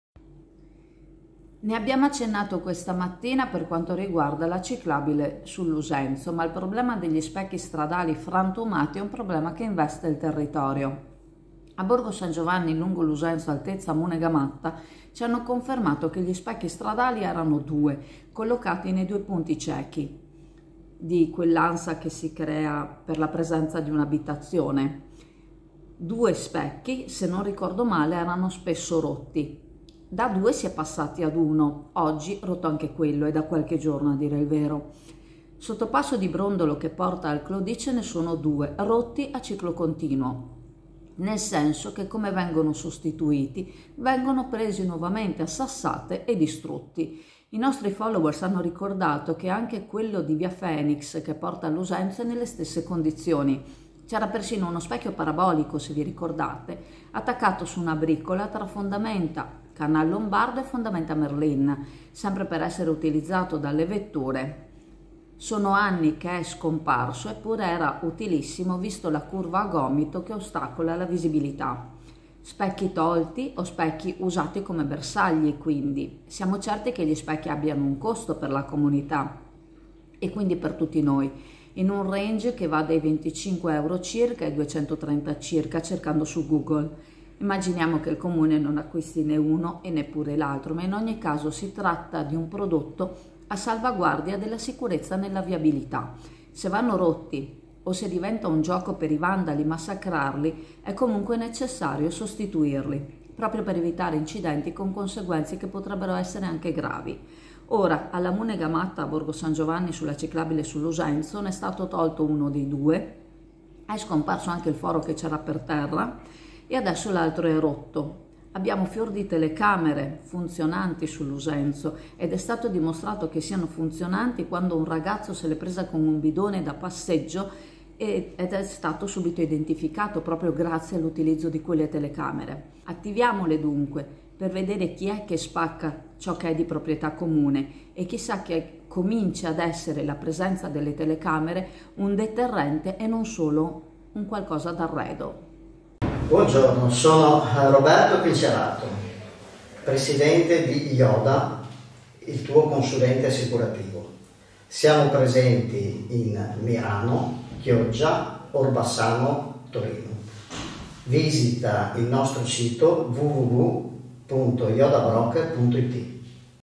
FRANTUMATO UN ALTRO SPECCHIO STRADALE A QUANDO LE TELECAMERE CONTRO IL VANDALISMO
di Micaela Brombo, Chioggia 7 ottobre 2024

Ne abbiamo accennato questa mattina per quanto riguarda la ciclabile Lusenzo, ma il problema degli specchi stradali frantumati è un problema che investe il territorio.
A Borgo San Giovanni, lungo Lusenzo, altezza Munega Matta, ci hanno confermato che erano due, collocati nei due punti ciechi, se non ricordo male spesso rotti. Da due si è passati a uno, oggi rotto anche quello.
Sottopasso di Brondolo che porta al Clodì, ce ne sono due, rotti a ciclo continuo . Nel senso che come vengono sostituiti vengono presi nuovamente a sassate e distrutti. I nostri followers hanno ricordato che anche quello di Via Fenix  che porta al Lusenzo è nelle stesse condizioni. C’era persino uno specchio parabolico attaccato su una bricola tra Fondamenta Lombardo e Fondamenta Merlin, sempre per essere utilizzato dalle vetture. Sono anni che è scomparso eppure era utilissimo visto la curva a gomito che ostacola la visibilità.
Specchi tolti o specchi usati come bersagli quindi. Siamo certi che gli specchi abbiano un costo per la comunità, e quindi per tutti noi, in un range che va dai 25 euro circa ai 230 circa cercando su Google. Immaginiamo che il comune non acquisti ne uno ne l’altro, ma in ogni caso si tratta di un prodotto a salvaguardia della sicurezza nella viabilità. Se vanno rotti, o diventa un gioco per i vandali massacrarli è comunque necessario sostituirli, proprio per evitare incidenti con conseguenze anche gravi.
Ora alla Munega Matta ne è stato tolto uno dei due, è scomparso anche il foro a terra e adesso l’altro è rotto. Abbiamo fior di telecamere funzionanti sul Lusenzo, è stato dimostrato quando un ragazzo se l’è presa con un bidone per il rifiuto da passeggio. Attiviamole anche per vedere chi spacca ciò che è di proprietà comune, chissà che la presenza delle telecamere cominci ad essere un deterrente.